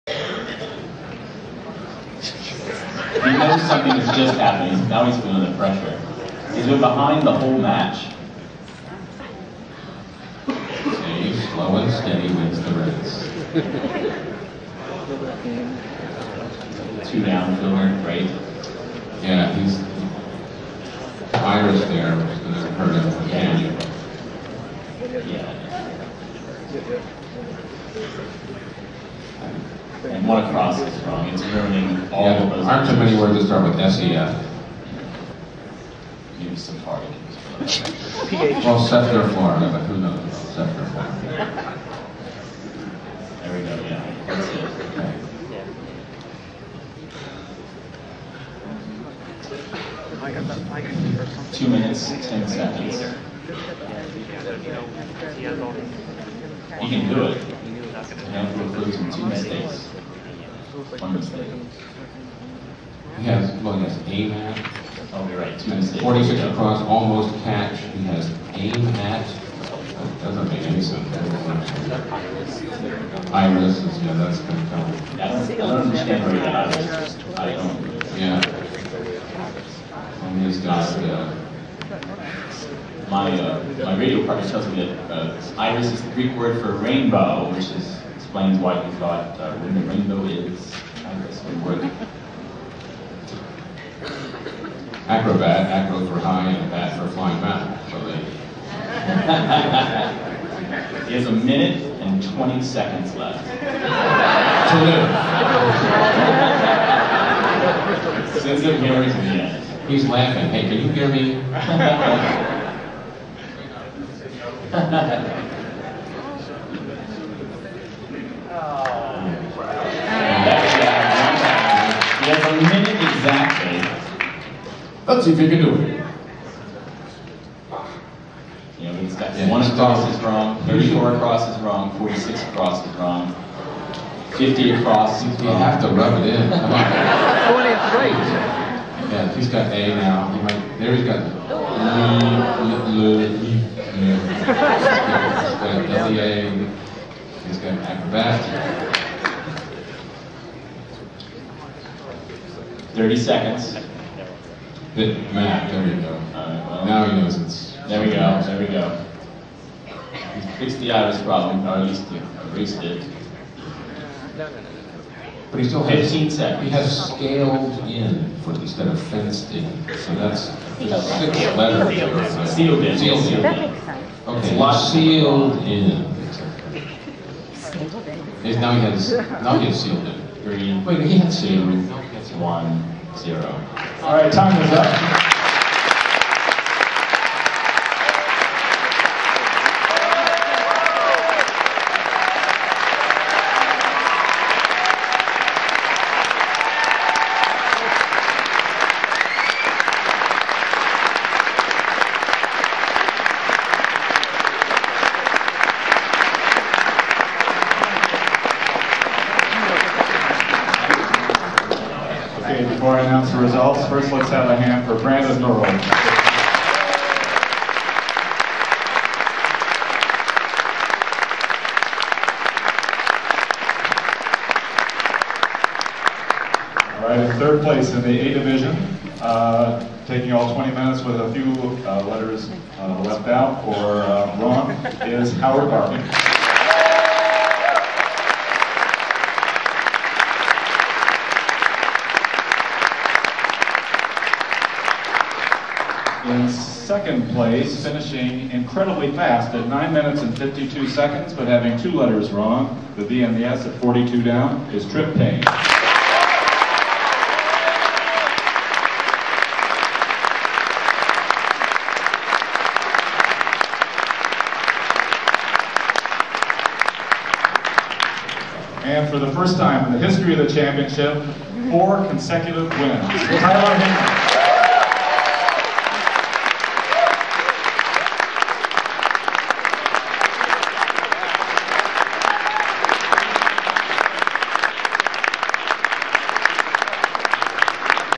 2008 American Crossword Puzzle Tournament "A" Finals Pt 3
March 3, 2008  Howard  Barkin gamely works on his puzzle.  Congratulations to the winner!  History is made!